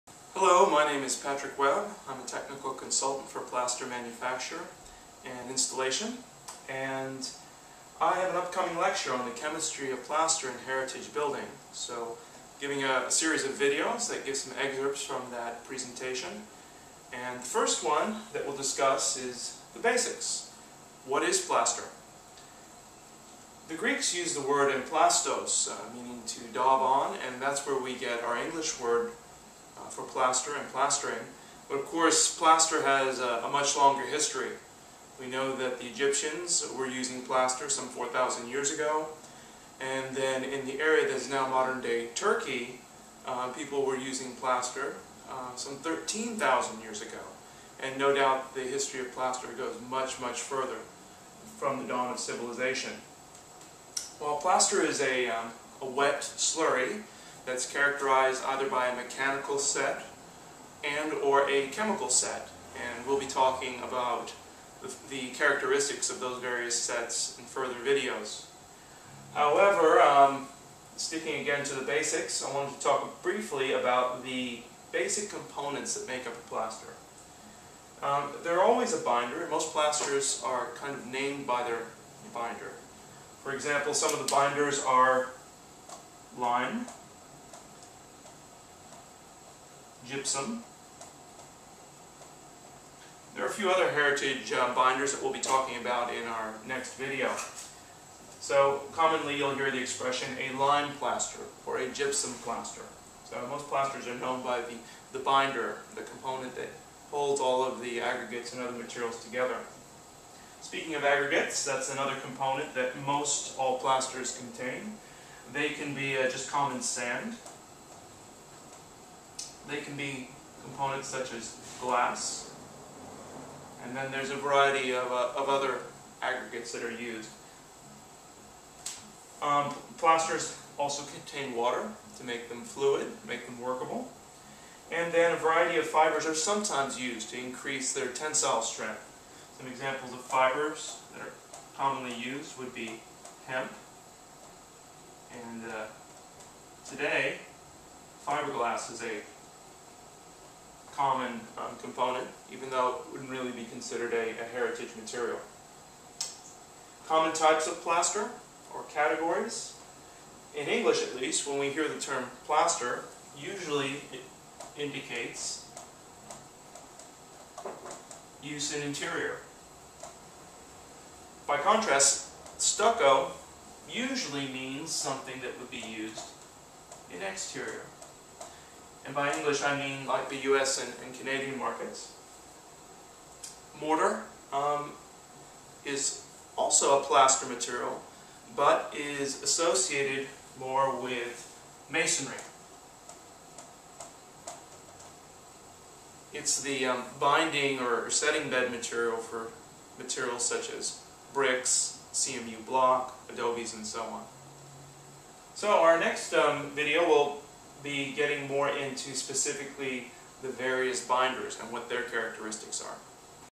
The Chemistry of Plaster in Heritage Building - Part 1 - PLASTER
PLASTER

This is the first in a series of videos exploring what is heritage plaster including the traditional binders, aggregates as well as appropriate specifications.